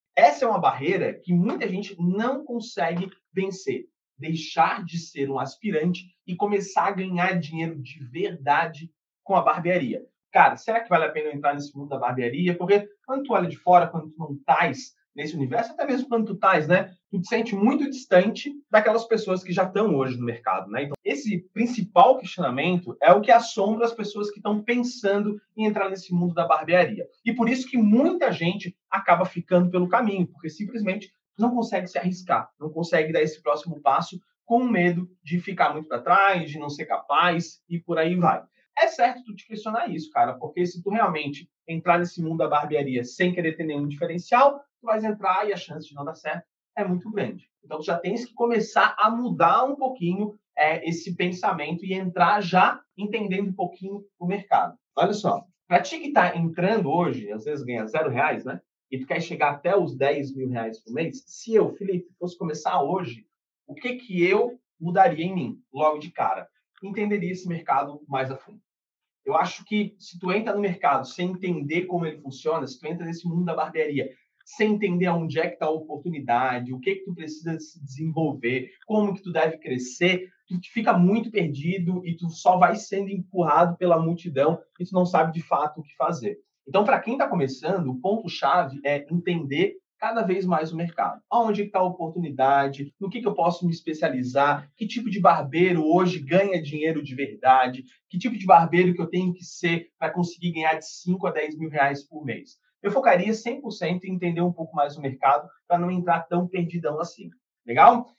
Como deixar de ser um aspirante a barbeiro e ganhar de 5 a 10k por mês
Fala galera! Tudo certo? Como deixar de ser aspirante a barbeiro e ganhar de 5 a 10k por mês, sendo um barbeiro profissional? Talvez você se faça muito essa pergunta. Nesse vídeo eu vou falar sobre alguns pontos que é preciso fazer para dar esse START na sua carreira e assim viver de barbearia. Confere aqui!